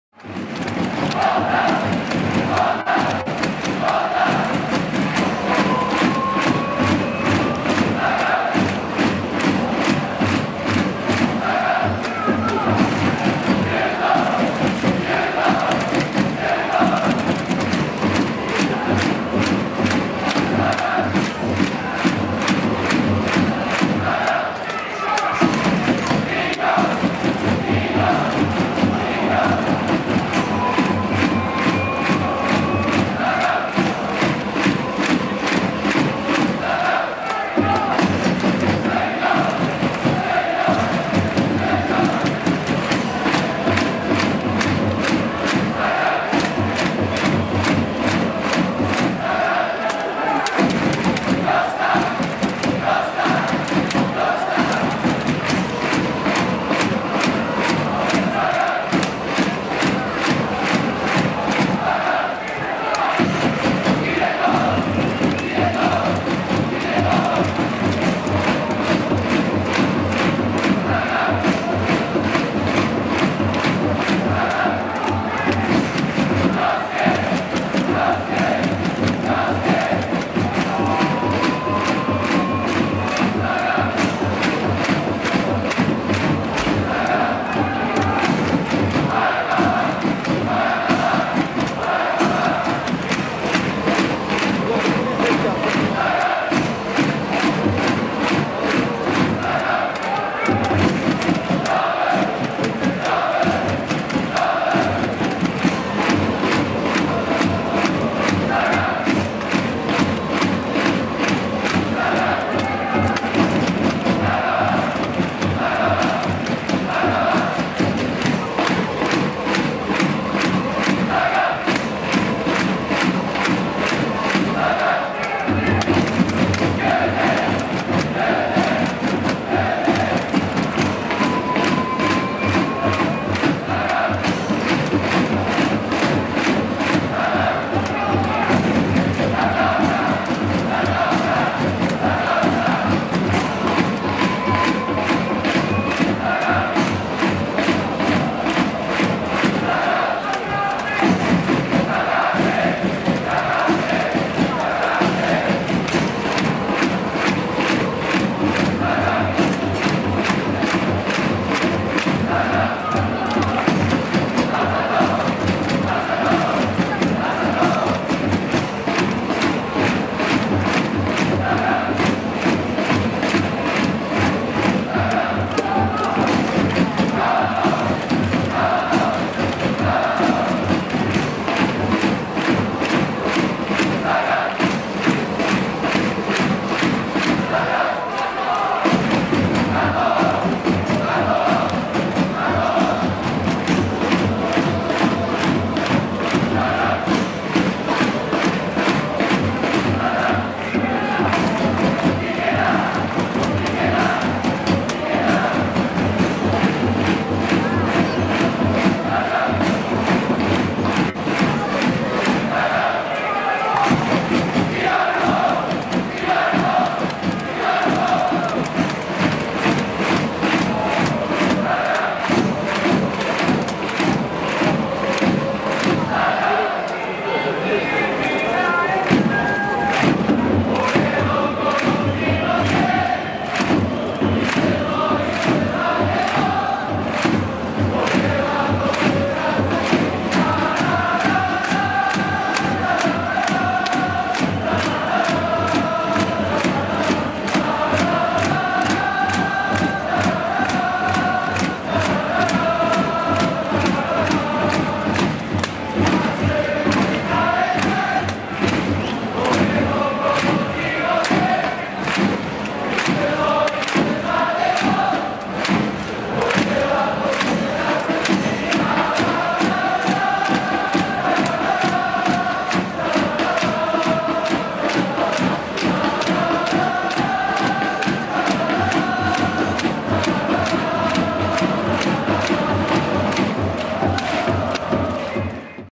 サガン鳥栖 各選手コール 2018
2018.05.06　J1第13節 サガン鳥栖vs清水エスパルス
試合前の各選手コール

【関連動画】
サガン鳥栖 各選手チャント集 2018